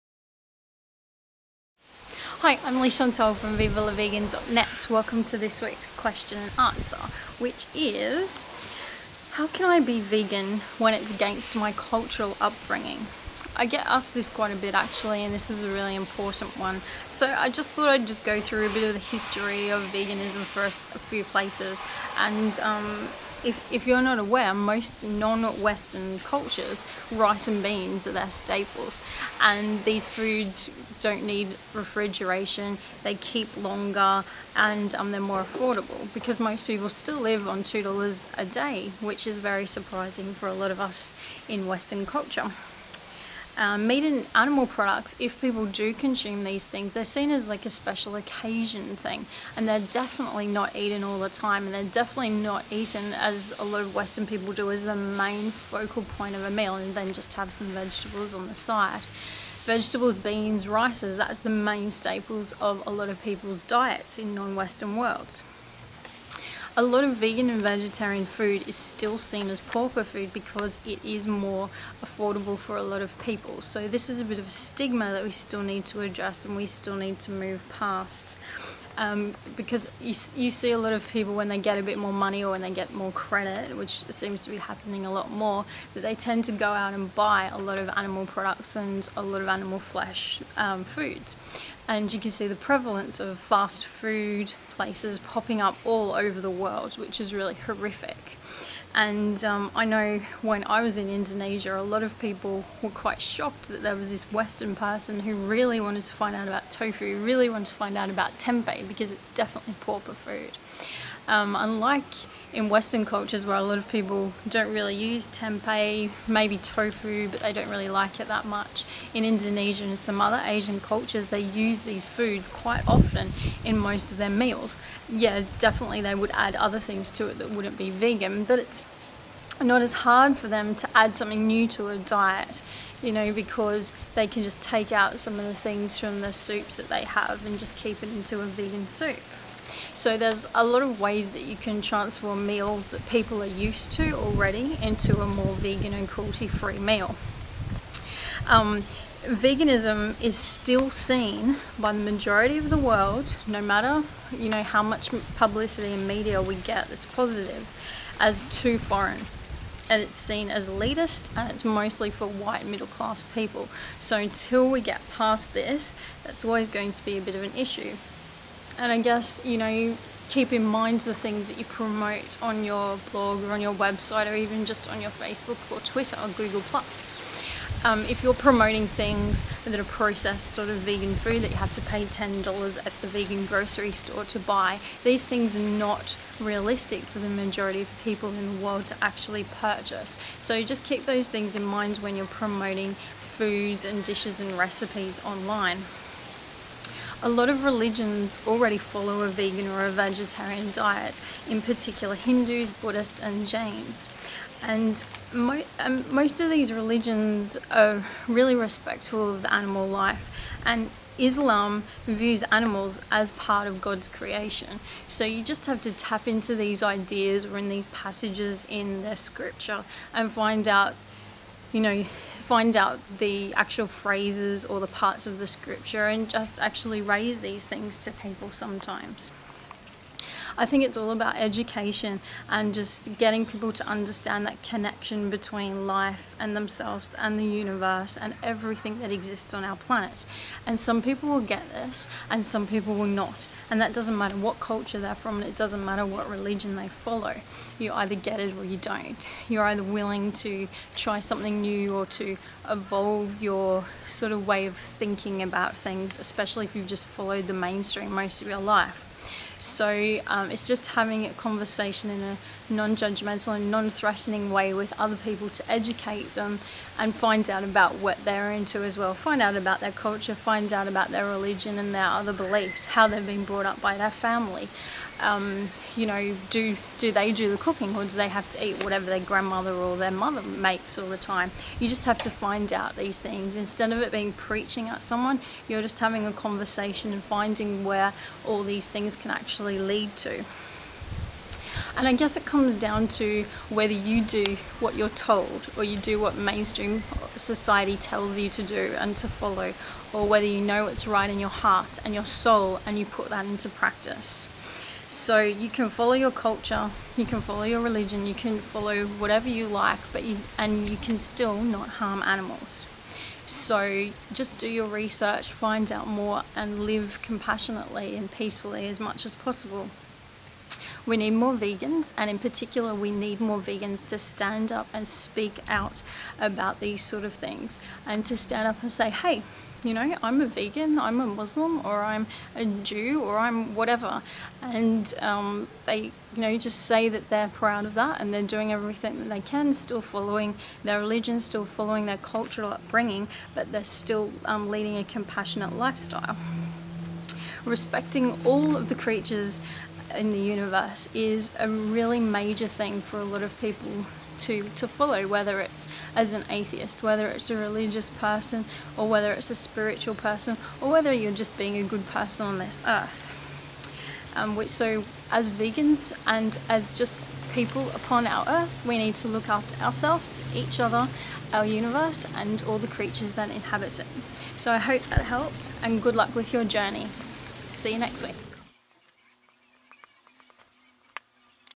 Viva la Vegan! Q&A series: How can I be Vegan when it's against my Cultural Upbringing?
Leigh-Chantelle from Viva la Vegan! explains how you can be vegan when your cultural upbringing makes it hard.